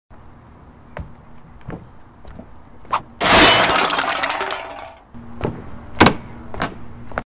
Title Card: Bill
Borderlands-style title card for some random nerd I found.

This is a test for seeing if this can be done in a film we're working on. I think it worked great! I used Photoshop to split up the layers and paint out mistakes, After Effects to animate the layers, and Premiere to put it all together. FYI the background is NOT from the same frame as the human; you can occasionally see past the animated body, so I had to find a clean frame to put back there.